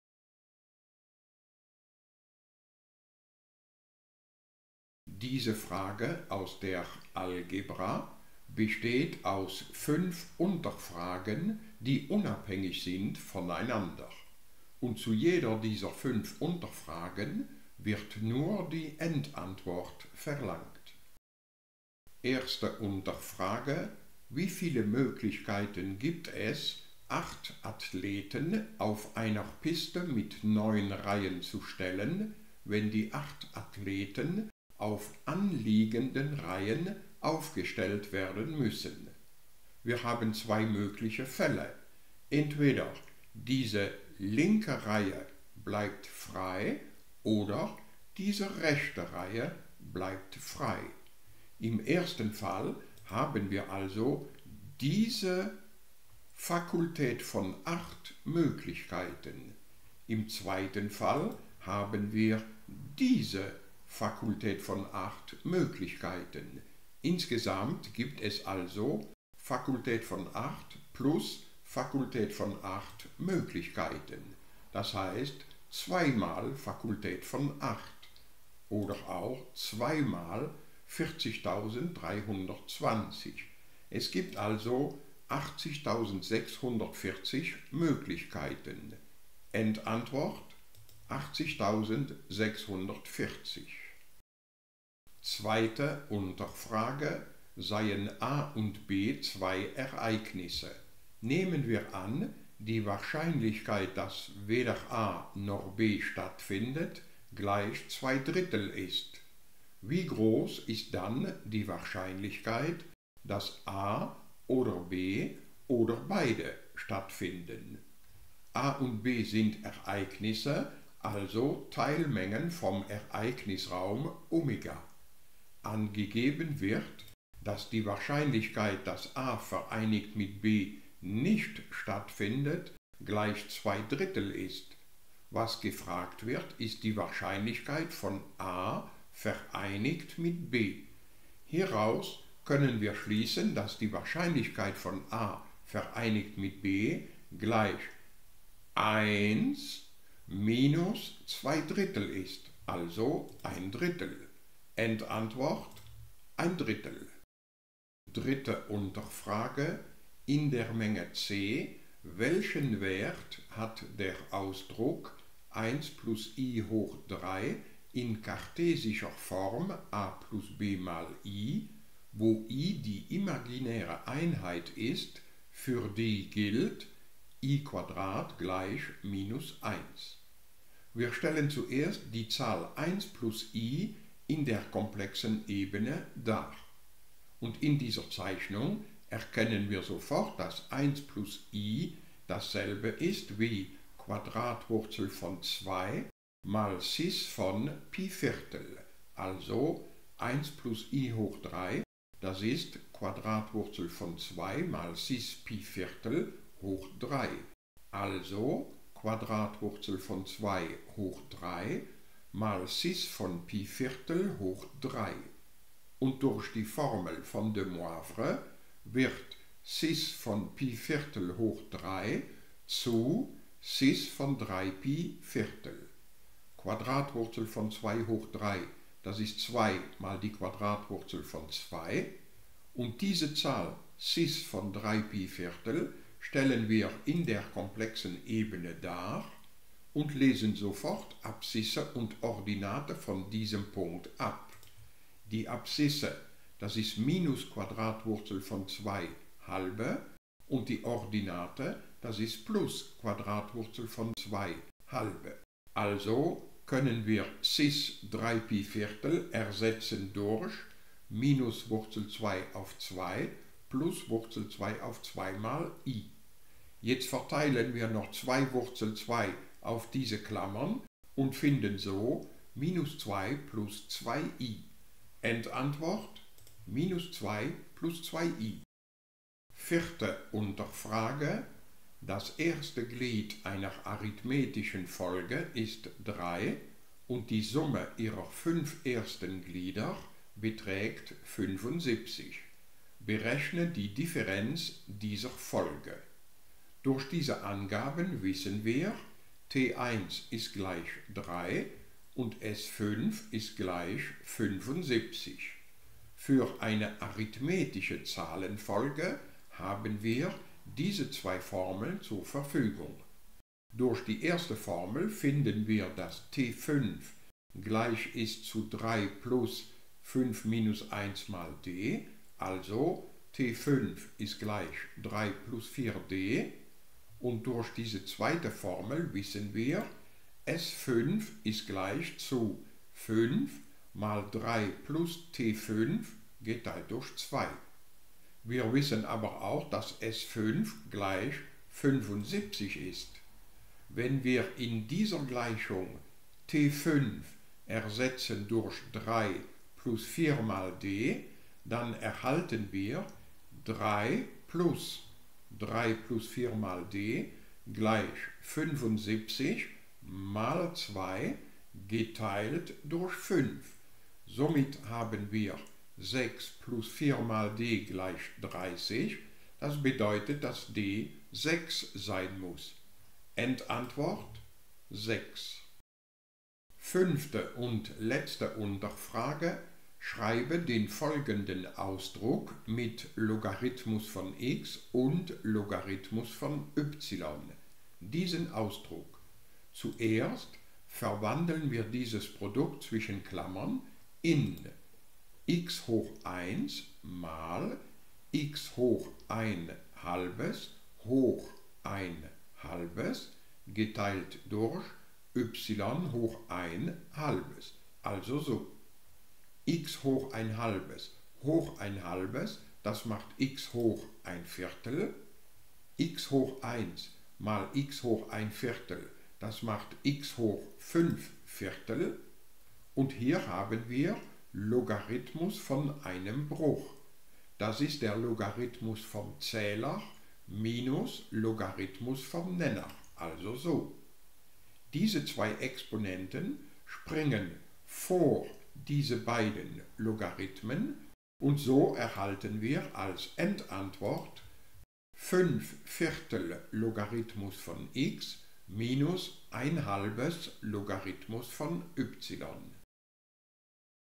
Aufnahmeprüfung Universität Lüttich - Algebra 024
Wir beantworten die erste Frage zur Algebra, die im Juli 2024 an der Universität Lüttich bei der Aufnahmeprüfung Zivilingenieur gestellt wurde.